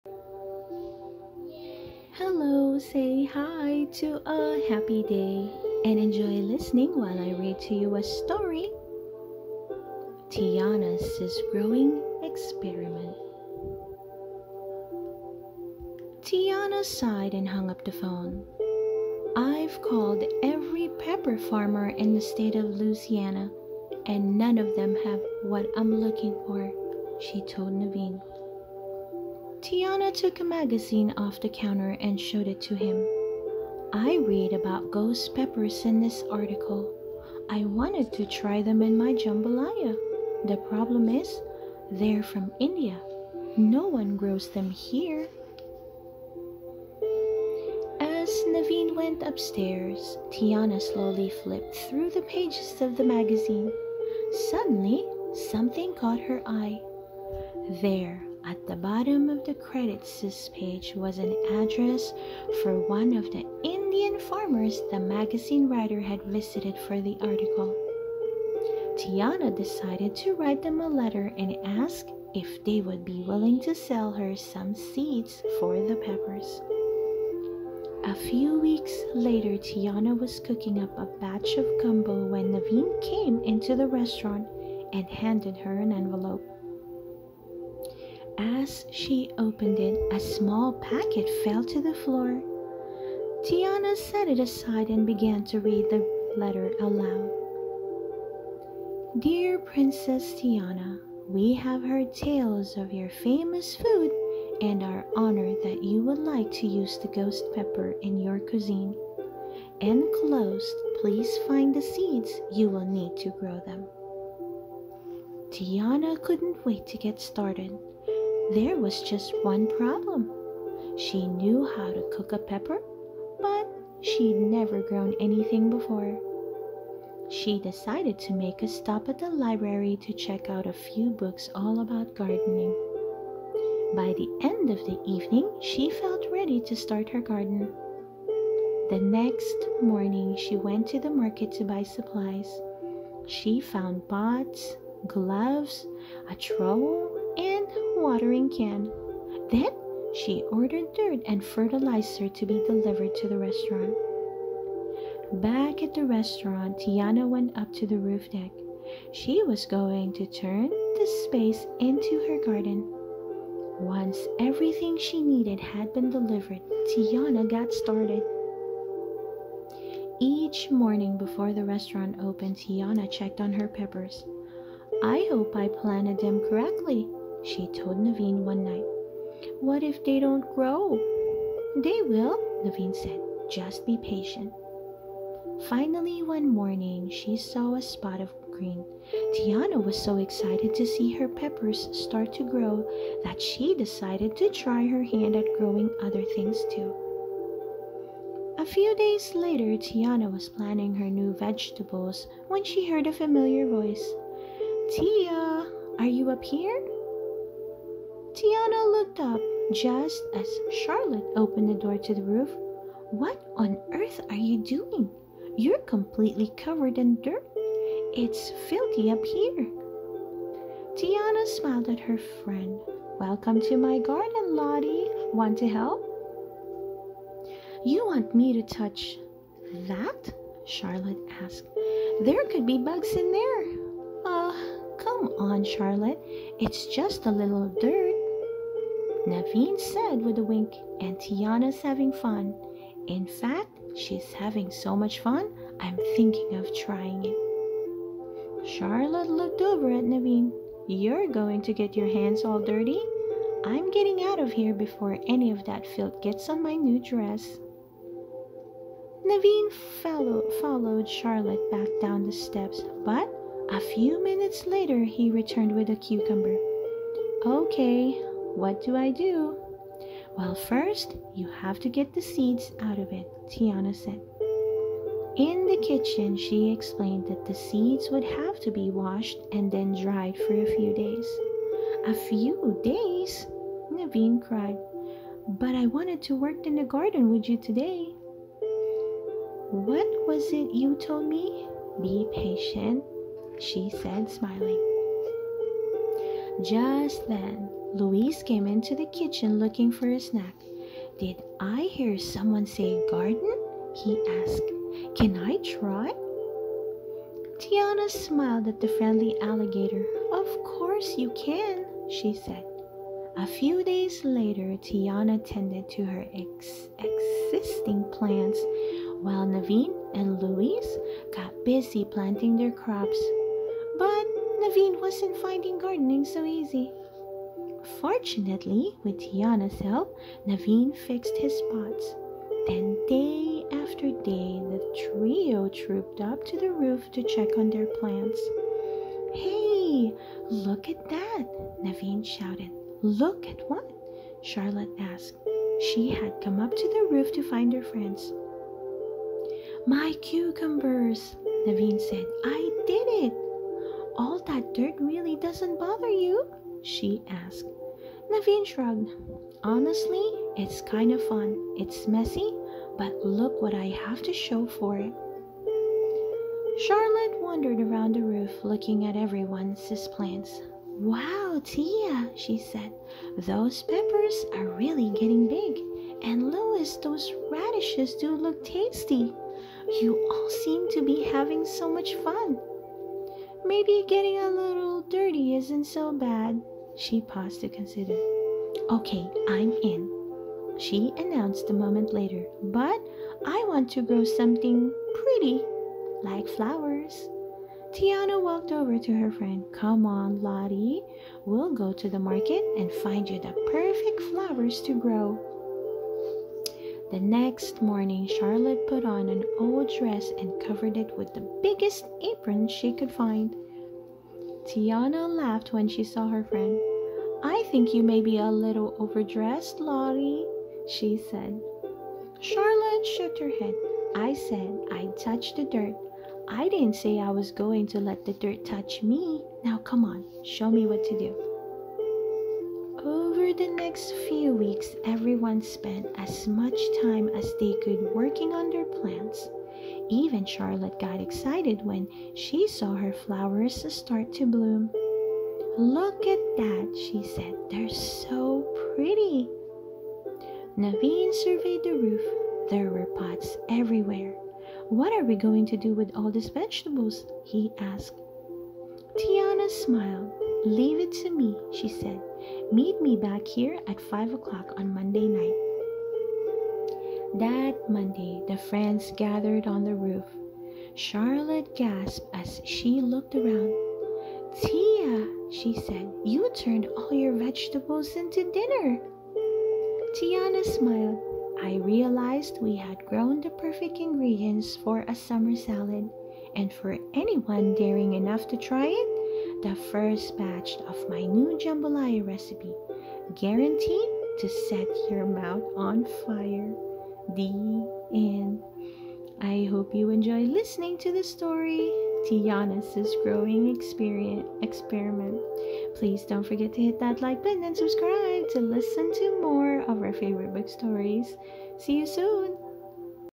Storytelling | Tiana's Growing Experiment | The Princess and the Frog
The Disney Stories: Tiana's Growing Experiment

Embark on all-new adventures in a collection featuring the Disney Princessess